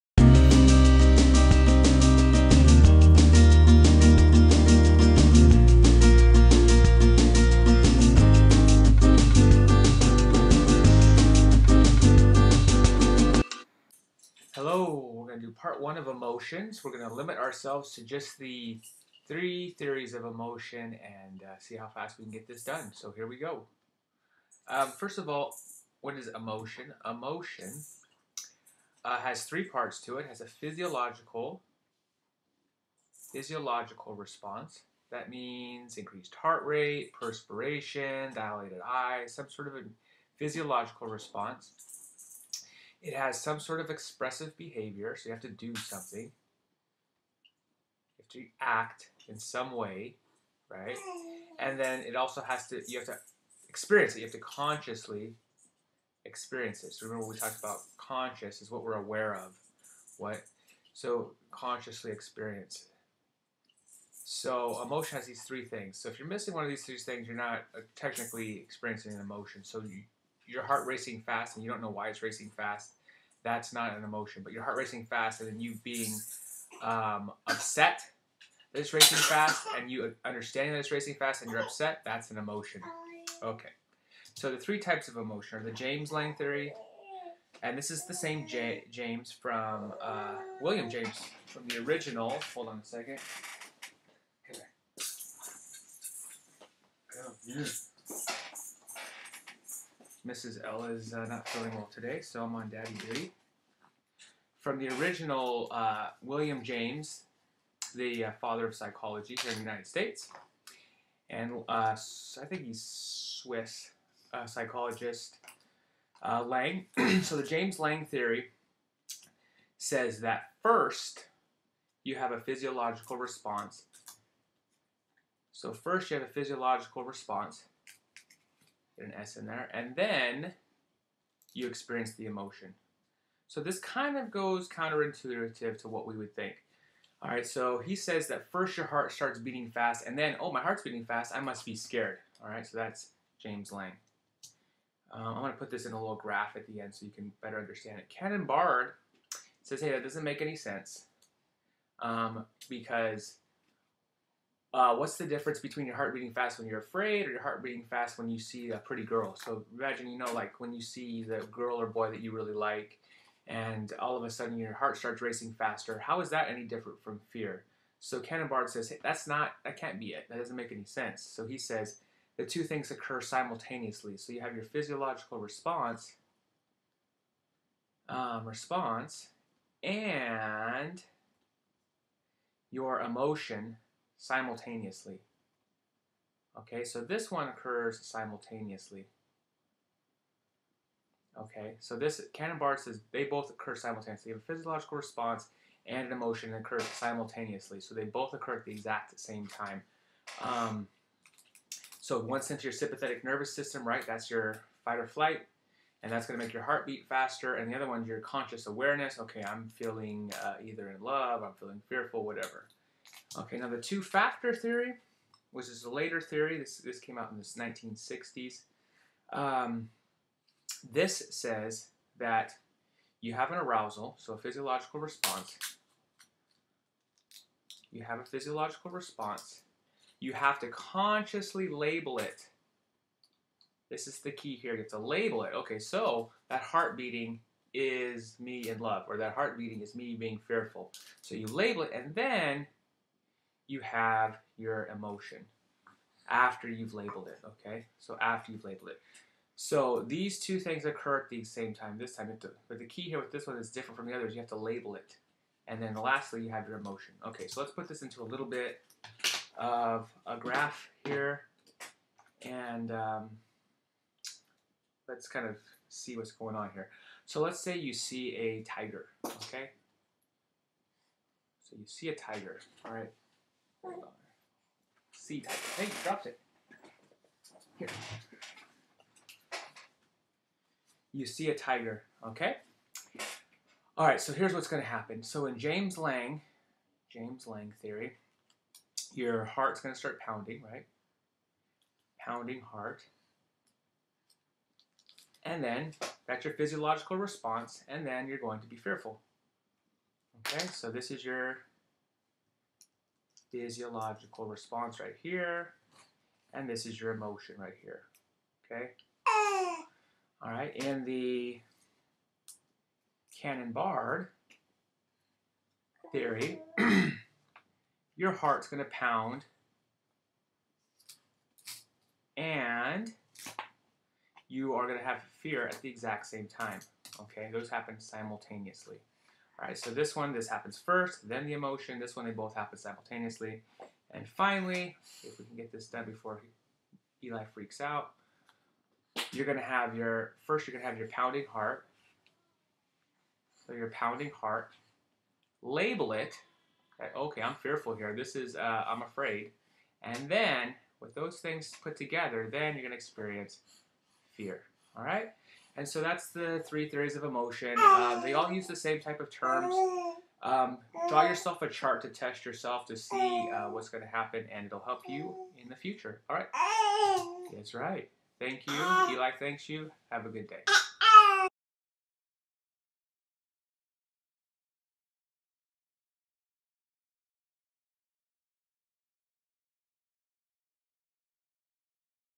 AP Psychology - Emotions - Part 1 - Theories of Emotions
An explanation of the three major theories of emotions in psychology. 366-368